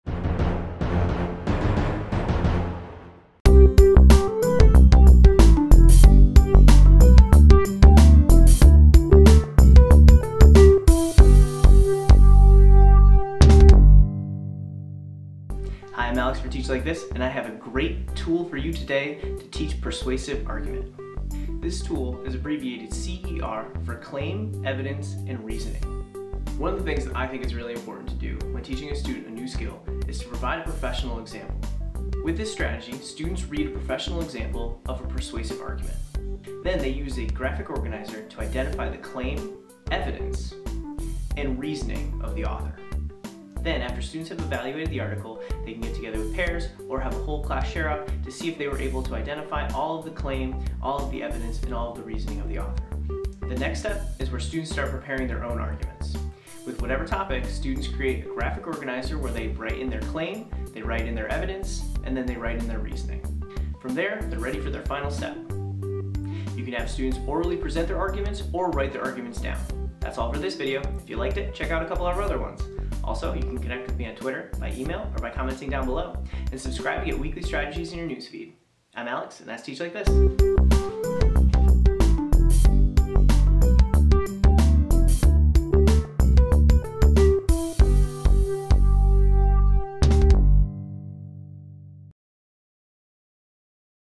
How to use C.E.R. - TeachLikeThis
Video #32 C.E.R.

This strategy teaches persuasive writing and speaking. In the first part, students look at a professional argument and identify the CLAIM, EVIDENCE and REASONING of the author. Then, when developing their own arguments, students fill in their CLAIM, EVIDENCE and REASONING in a graphic organizer before developing their essay or speech.